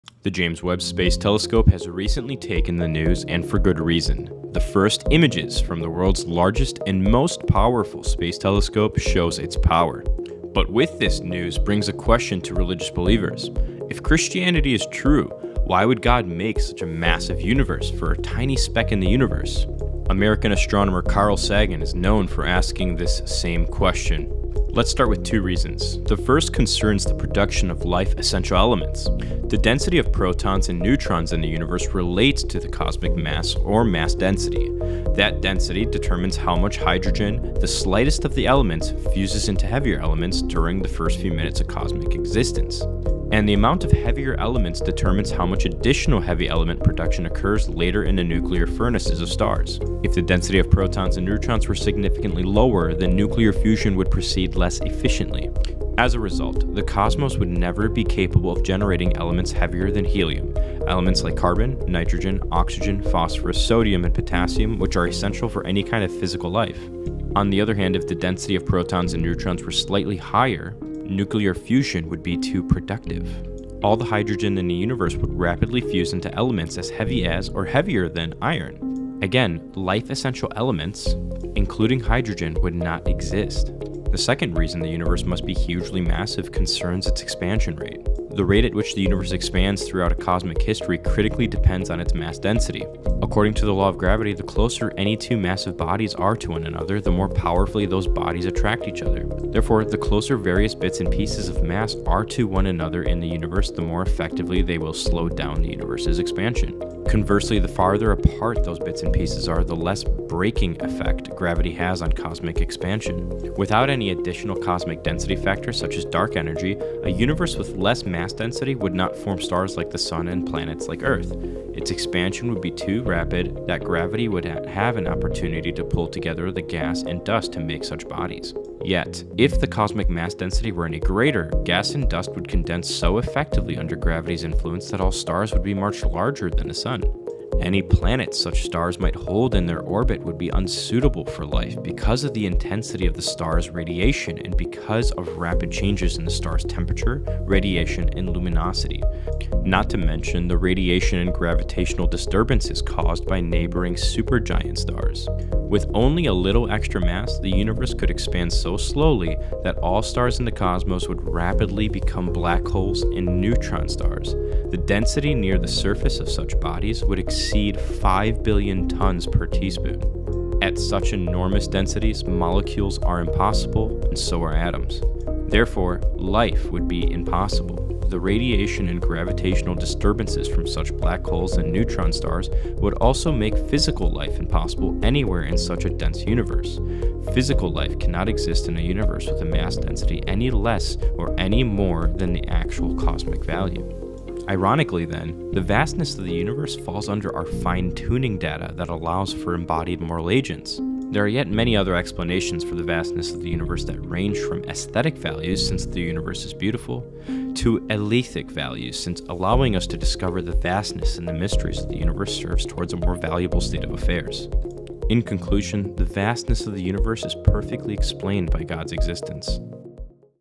James Webb Space Telescope & God
The James Webb Space Telescope has been taking the news recently. Here is a short video on a resurfacing objection people have been giving to Christianity in light of that.